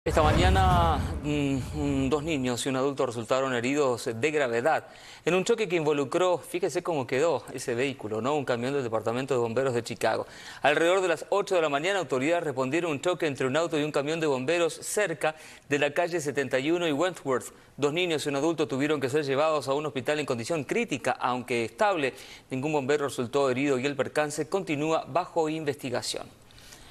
Dos menores y un adulto salen heridos de un accidente en Chicago
El choque ocurrió en la intersección de las calles 71 y Wentworth.